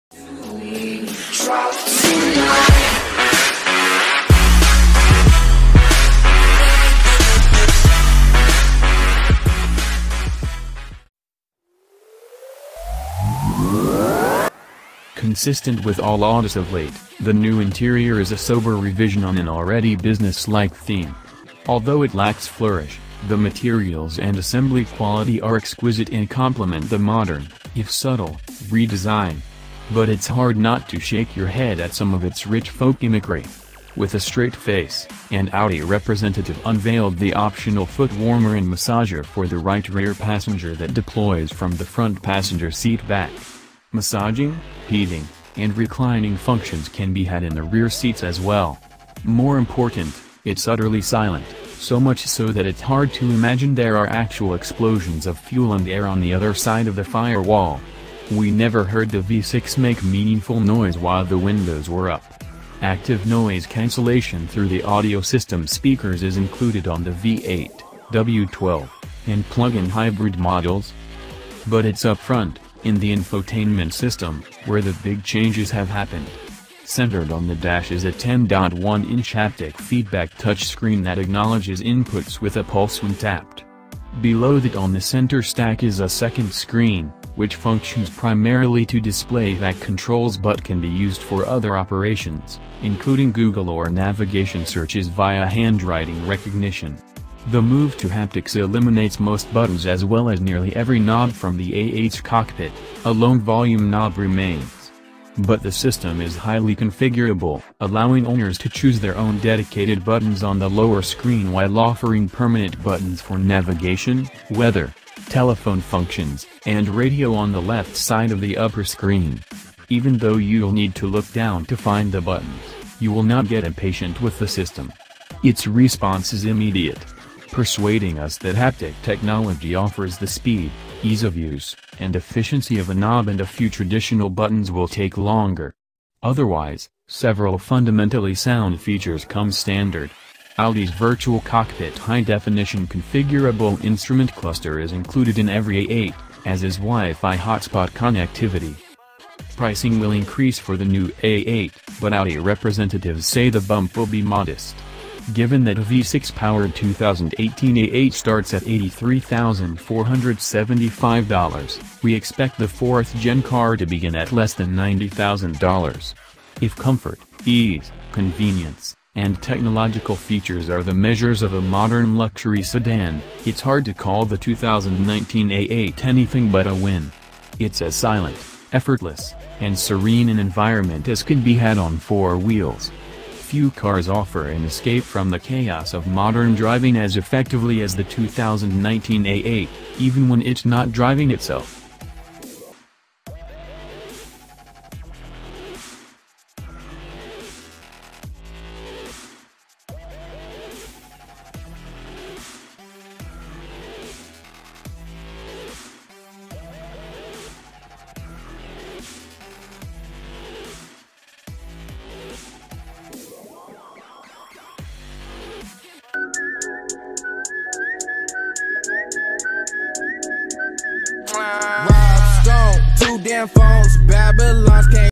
HOT!!!! NEW 2019 AUDI A8
HOT! NEW 2019 AUDI A8. Pricing will increase for the new A8, but Audi representatives say the bump will be modest. Given that a V-6–powered 2018 A8 starts at $83,475, we expect the fourth-gen car to begin at less than $90,000. If comfort, ease, convenience, and technological features are the measures of a modern luxury sedan, it’s hard to call the 2019 A8 anything but a win. It’s as silent, effortless, and serene an environment as can be had on four wheels. Few cars offer an escape from the chaos of modern driving as effectively as the 2019 A8—even when it’s not driving itself.